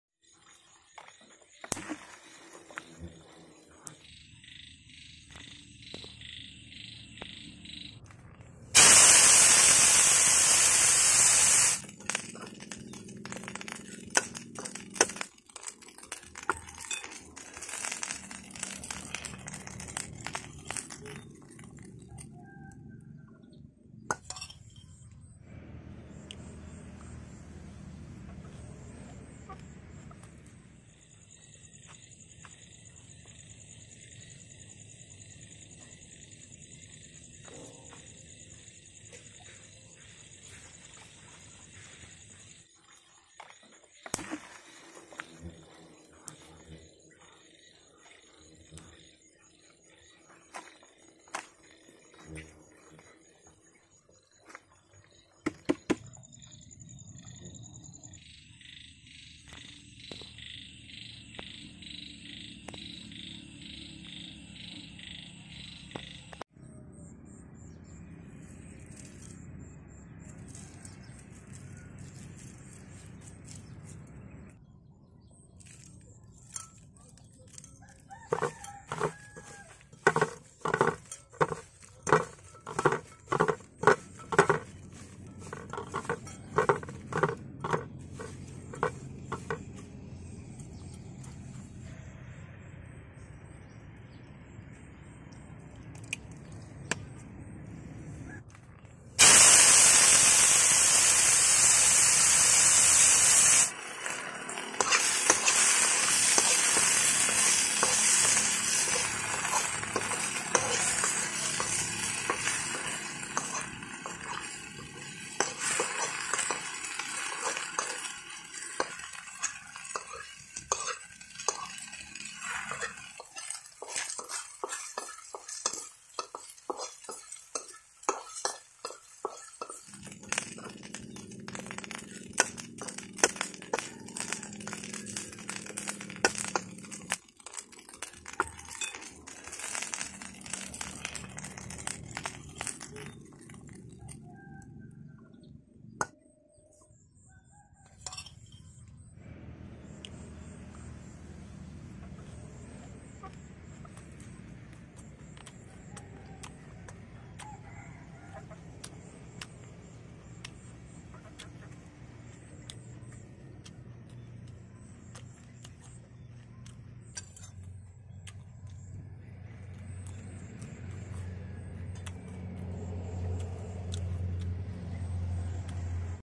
Delicious fry junebugs. food cooking June bugs
where ever we call them june beetles, or junebugs or may beetles, then small redish-brown, clumsy flyers can be annoying. they get their name because of when they emerge. so this tiny animal we can cook them for foods. some countries like some Cambodian eat them for food all so.
let's watch this video for more about cooking this animal ( June bugs ).